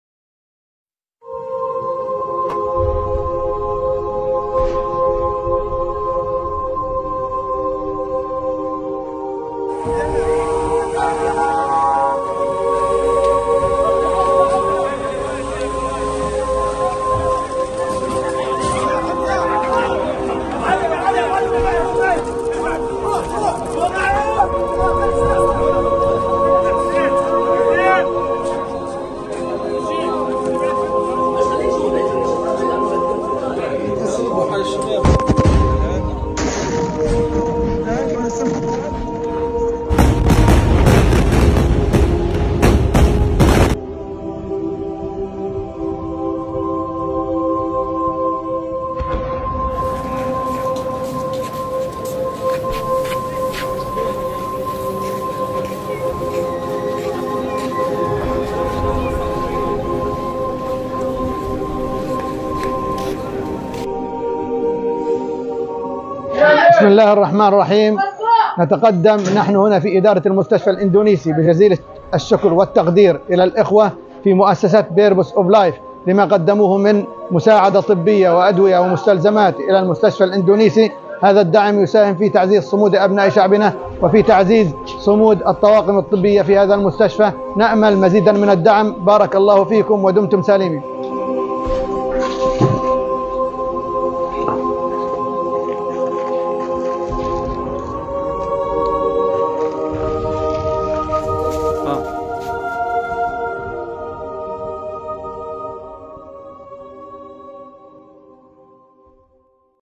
GAZA - MORE LIFE SAVING MEDICAL AID DELIVERED! BATCH 39
The struggle continues for the Palestinians and so does the aid from Purpose of Life!

Please keep supporting this blessed cause.